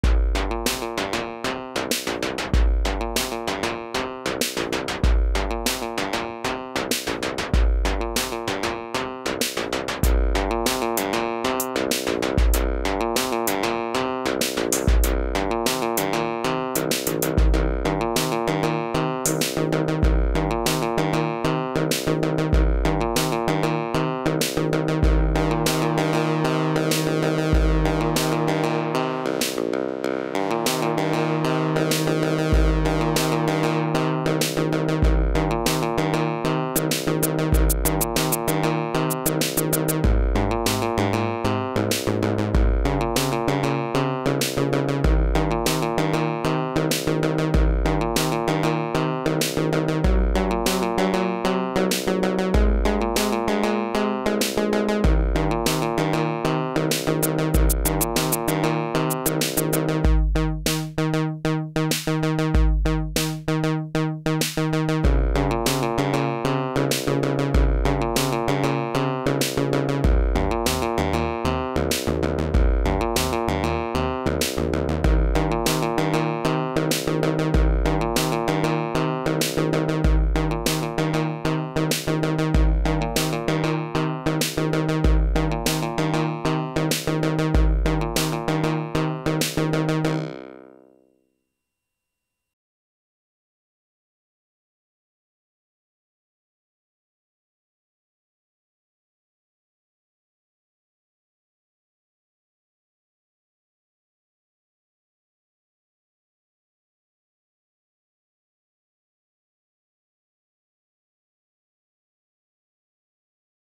Free the VSTs
Free VSTs that are as good or better than Reason Studio's ID8 presets, Grand, Dance, Digital, Organ, Guitars, Brass, Strings and 808 drums